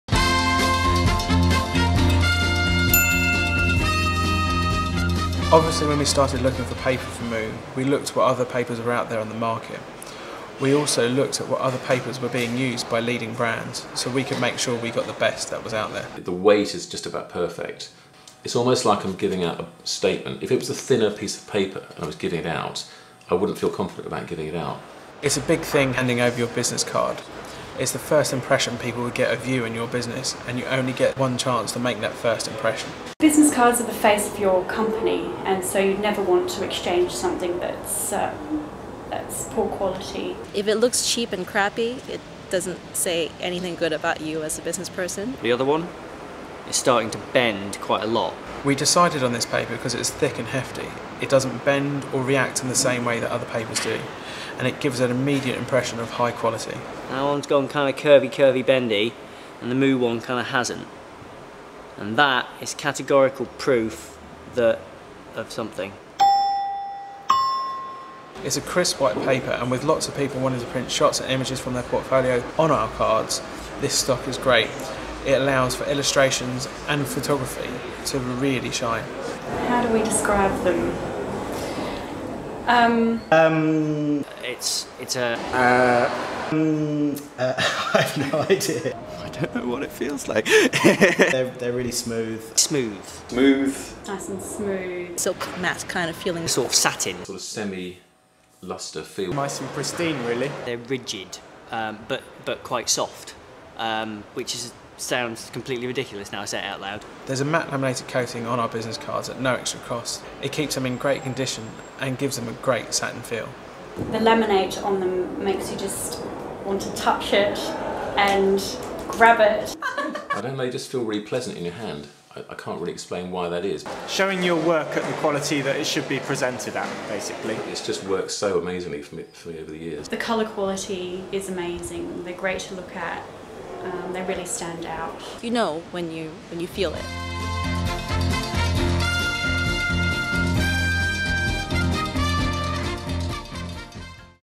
MOO Spotlight | Classic Paper
Sometimes it's hard to describe exactly how something feels. MOO's Business Cards and other products are printed on premium paper stock, with a smooth matte laminate. Although, we think there is more to it than that...